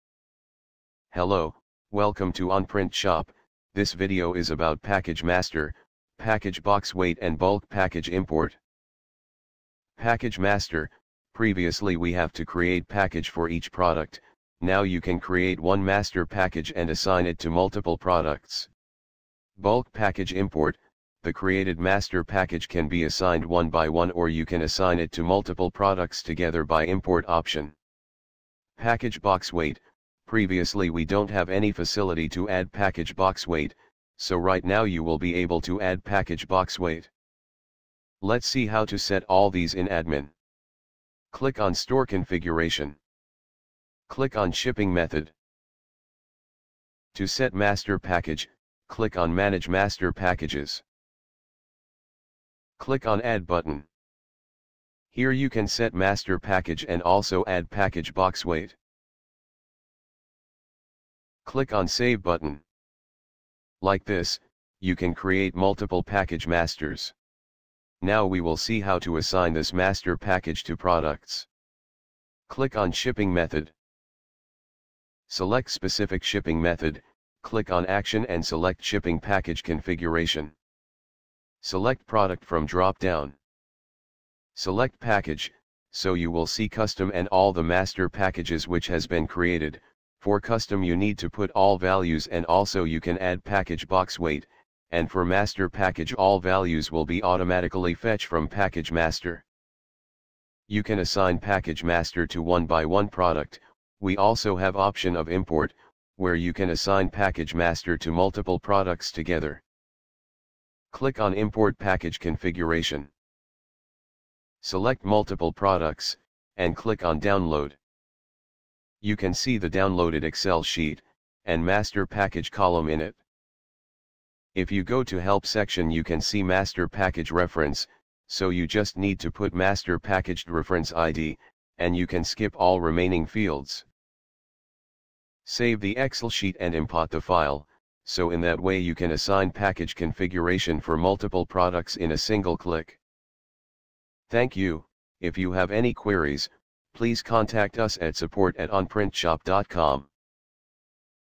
Package Master, Package Weight and Bulk Package Import | v7.2
Configure standard and common package for all products called the master package. We have also provided import packages in bulk instead of creating a package for every individual product one by one. Set package box weight, which will be utilized in the calculation of package weight and decide the package for the product(s) or an order.